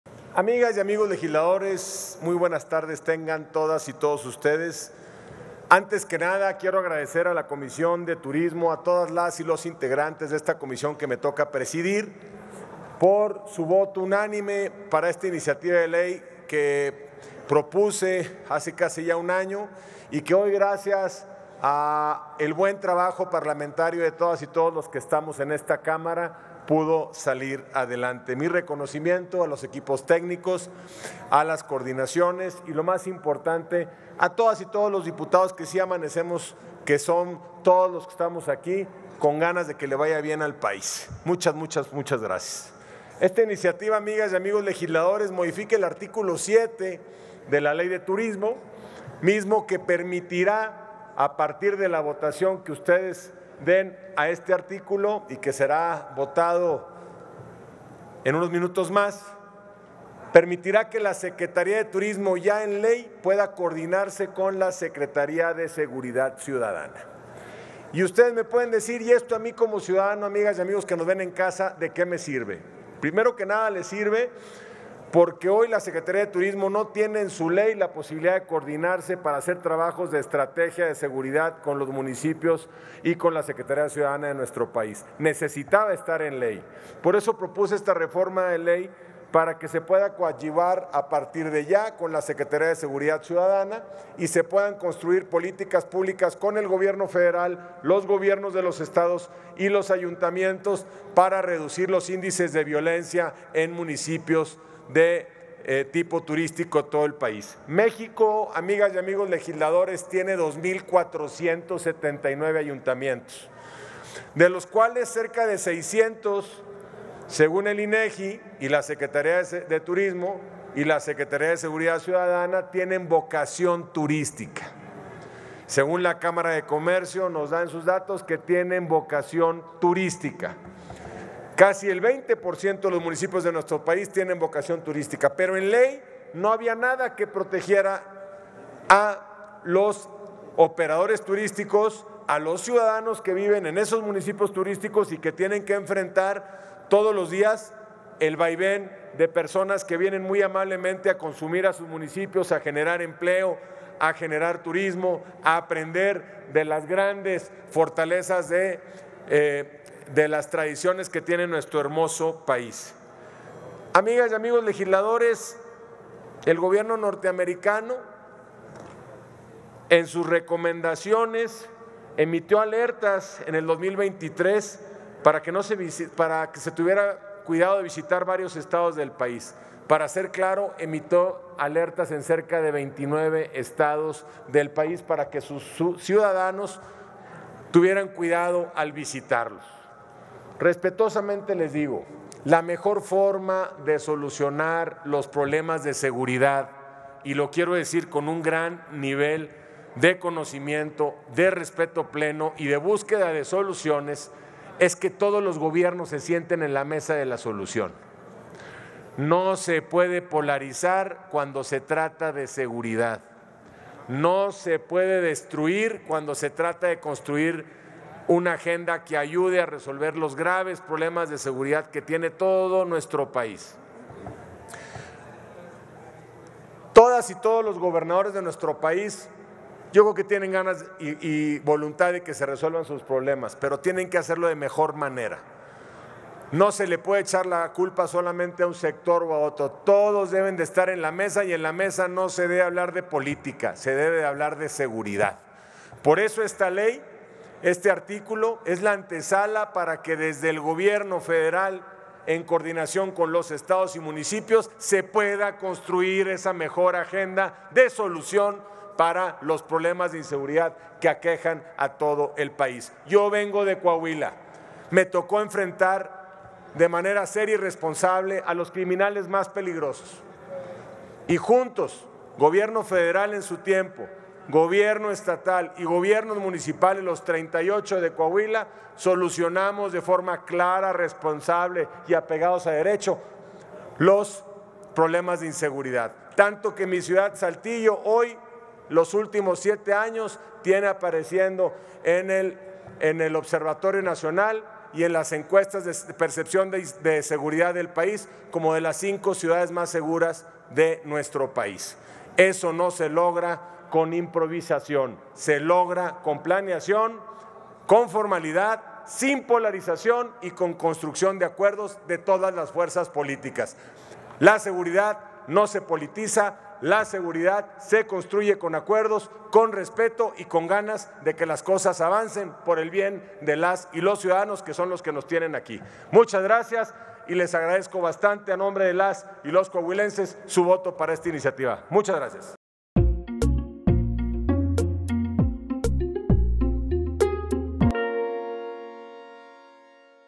Dip. Yerico Abramo Masso (PRI) /Fundamentación de dictamen
El diputado Yerico Abramo Masso (PRI) fundamenta el dictamen que reforma la fracción IX del artículo 7 de la Ley General de Turismo, con el objetivo de fortalecer la seguridad en los destinos turísticos de México.